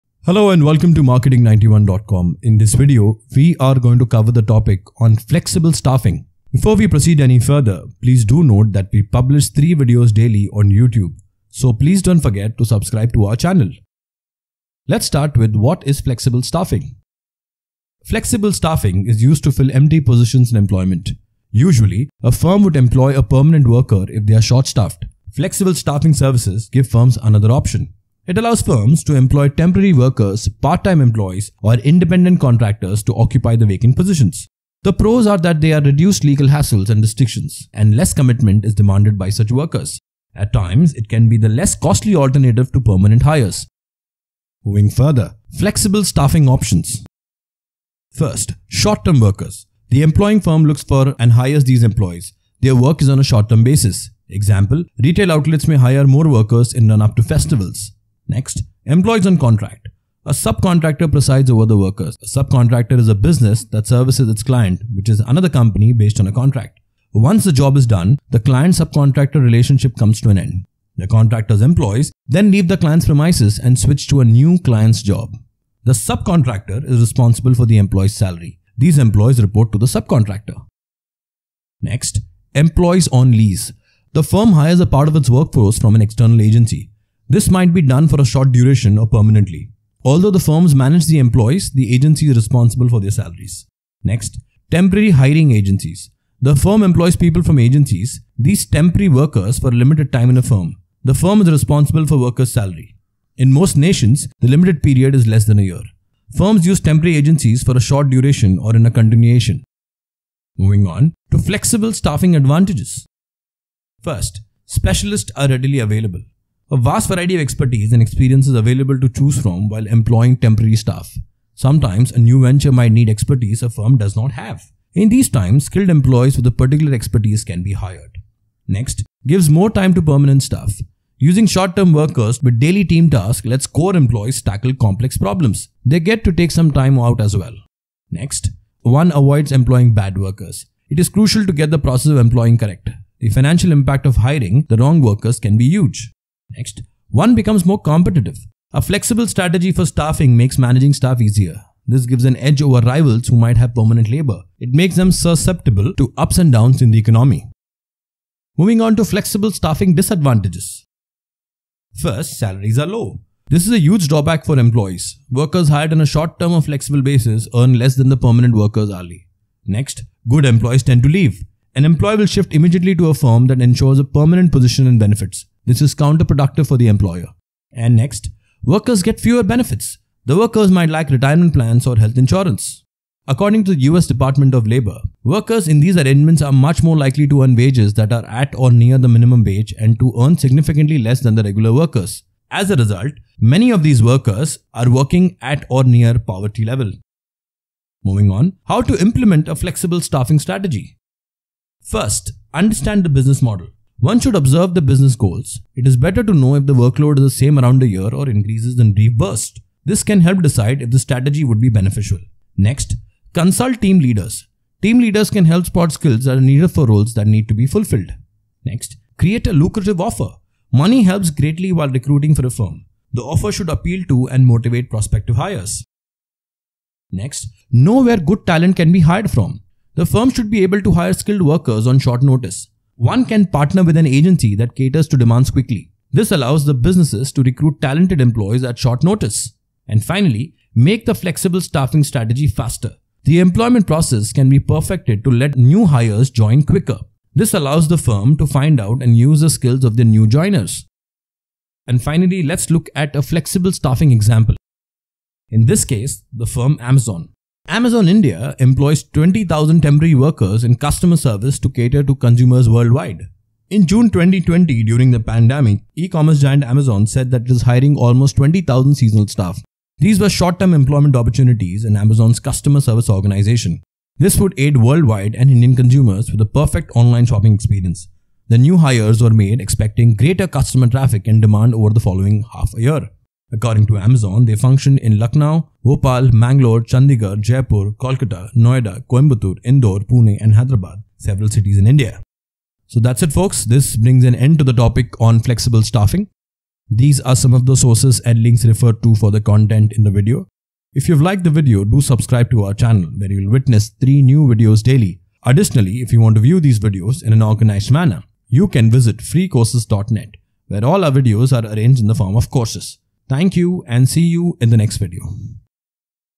What is Flexible Staffing? Examples, Advantages and Strategy of using Flexible Staff
Flexible Staffing refers to the option of filling up human vacancies in an organization with
 a part-time workforce, independent contractors, or temporary employees.

Flexible Staffing is a very common practice when organizations do not want to hire permanent employees and stay away from the related legal requirements

Flexible Staffing Options

1. Short-term workers

These workers hold implicit or explicit contracts of employment which is more than the one for a casual worker

2.  Employees on contract

These individuals are hired for a particular project and for a specific timeframe

3.  Employees on lease

It is an arrangement between a staffing agency and an organization where the former provides workers temporarily.

4.  Temporary hiring agencies

These are agencies that supply temporary employees to firms in need of seasonal or part-time workers.

Flexible Staffing Advantages

- Organizations have a vast network of experienced talent to choose from.

- Hiring flexible staffing helps core employees to reduce their work pressure
- An organization can easily dismiss a wrong fit or an incorrect choice.

- Provides the firm with a competitive advantage over firms that have only permanent staff

Flexible Staffing Disadvantages

- The salary is lower than permanent staff.

- They are not eligible for additional benefits offered to a permanent employee.

- Lack of retirement plan and health insurance can urge flexible staffing to leave the job midway.

How to implement a Flexible Staffing Strategy?

1. Understand your needs and know whether flexible staffing is the need of the hour
2. Consult team leaders to know about the requirements
3. Make the offer lucrative so that you can hire the best talent for the job
4. Be aware of the agencies which supply good talent
5. Make sure that the new hire can join quickly to utilize their skills

Example

1. Amazon

This video is on Flexible Staffing and it has the following sub-topics.

Time Stamps

0:00 Introduction to Flexible Staffing
00:21 What is Flexible Staffing?
00:52 Flexible Staffing Options
01:57 Flexible Staffing Advantages
02:44 Flexible Staffing Disadvantages
03:25 How to implement a Flexible Staffing Strategy?
04:18 Flexible Staffing Example